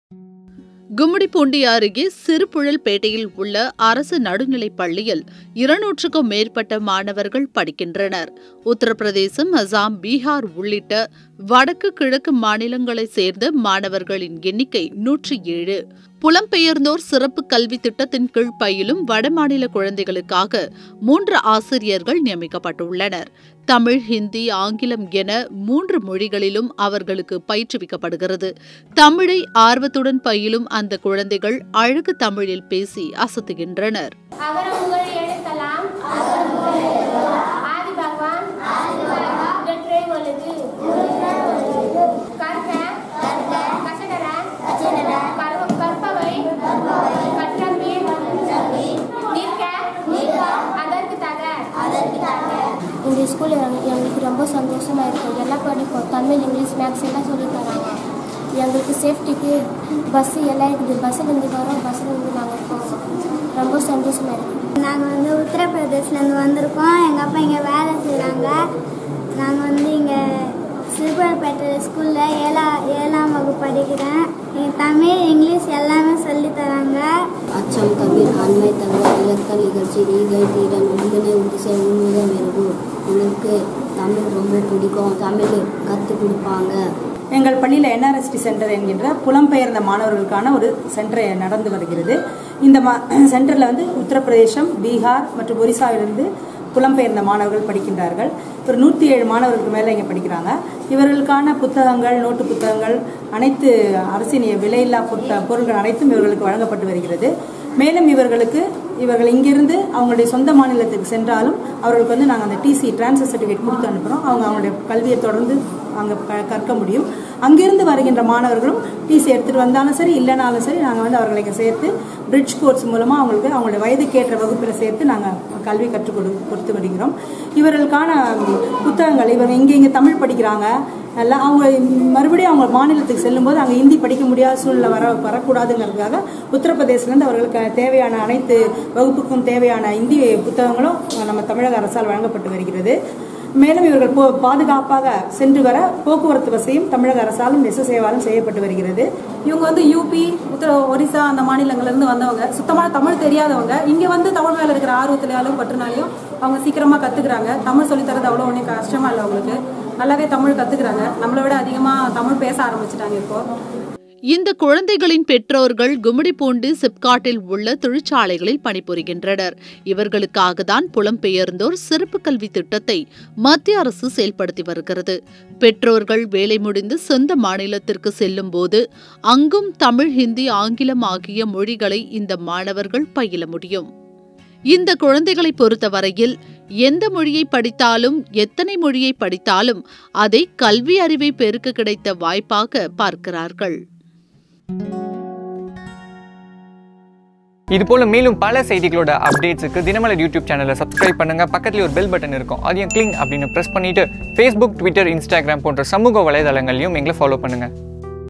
அழகுத்தமிழில் அசத்தும் வடமாநில குழந்தைகள் | North Indian Students in Tamil School
கும்மிடிப்பூண்டி அருகே
சிறுபுழல்பேட்டையில் உள்ள
அரசு நடுநிலைப்பள்ளியில்
200-க்கும் மேற்பட்ட
மாணவர்கள் படிக்கின்றனர்.
உத்தரப்பிரதேசம், அசாம், பீகார் உள்ளிட்ட
வடக்கு, கிழக்கு மாநிலங்களைச்சேர்ந்த
மாணவர்கள் எண்ணிக்கை 107.

புலம்பெயர்ந்தோர் சிறப்புக் கல்வி திட்டத்தின்கீழ்
பயிலும் வடமாநில குழந்தைகளுக்காக
3 ஆசிரியர்கள் நியமிக்கப்பட்டுள்ளனர்.
தமிழ் ,இந்தி ,ஆங்கிலம் என மூன்று மொழிகளிலும்
அவர்களுக்கு பயிற்றுவிக்கப்படுகிறது.
தமிழை ஆர்வத்துடன் பயிலும் அந்தக் குழந்தைகள்